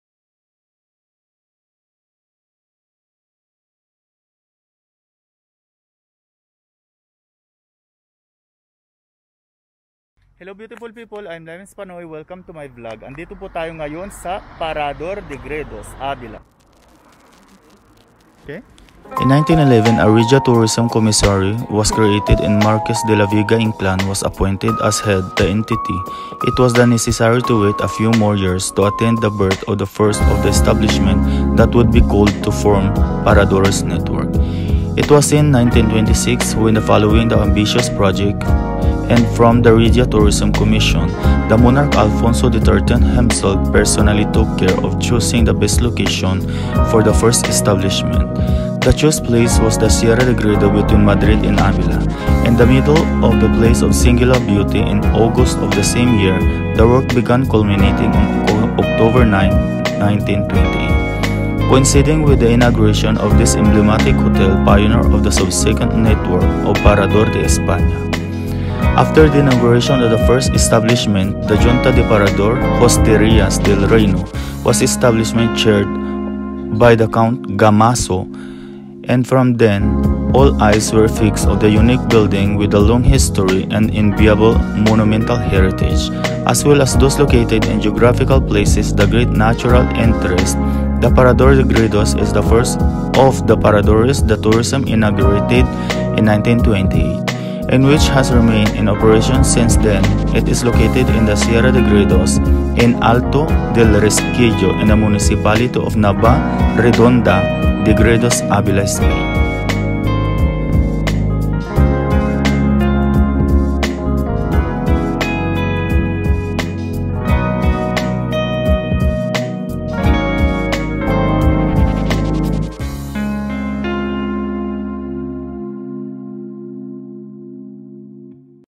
Parador de Gredos**** ll Sierra de Gredos ll Ávila ll Spain
The Parador de Gredos is a historic hotel located in the heart of the Sierra de Gredos, in the province of Ávila, in Spain. Located in a stunning natural environment, this parador offers guests a unique experience in the middle of mountains, valleys and forests. The building that houses the Parador de Gredos was originally a 12th century monastery and was later used as a royal residence.